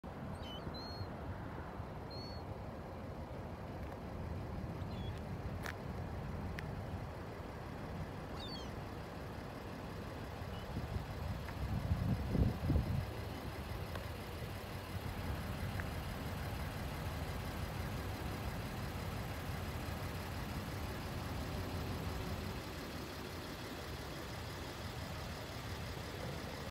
When your requested in Seattle and detour to check out Puget Sound.
Checking out the launch area in Puget Sound to plan a trip towing the OS up their.